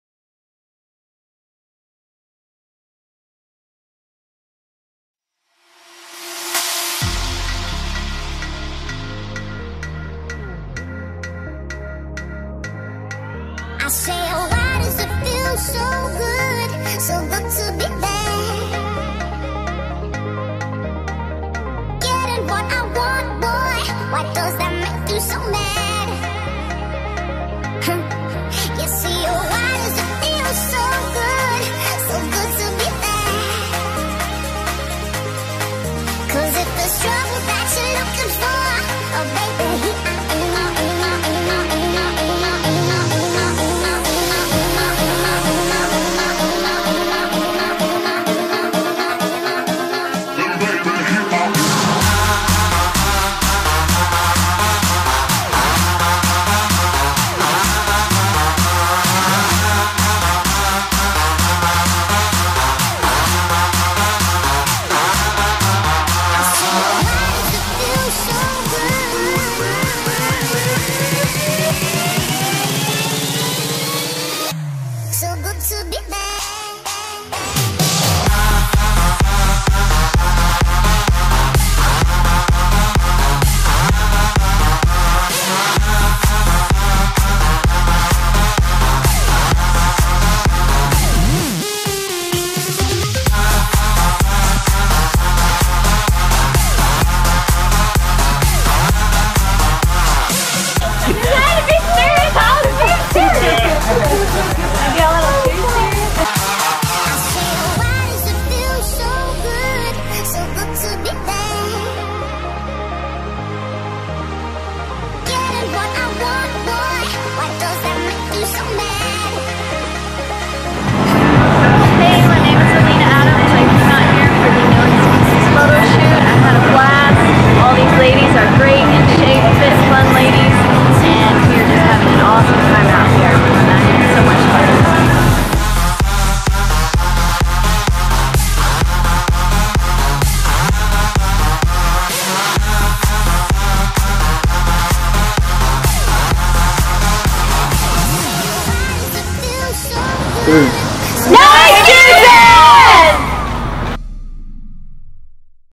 NES Behind The Scenes photoshoot
In just 7 days some of Houston's finest photographers, make-up artists and fitness athletes got together for an "impromptu" photo shoot. It was a spur of the movement event. This is the behind the scenes results of what happens when a group of positive people get together with a single-minded focus.